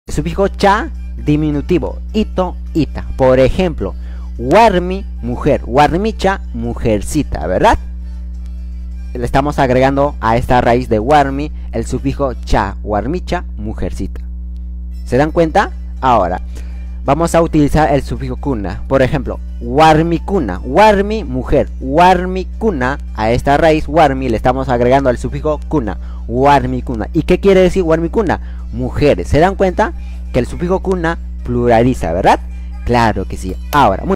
SUFIJO KUNA Y CHA EN QUECHUA/ Aprende Quechua con Wayna Tunki
Sufijos  KUNA y CHA en quechua. Sigamos difundiendo nuestro RUNA SIMI. Aprende quechua con Wayna Tunki

Primero lo nuestro.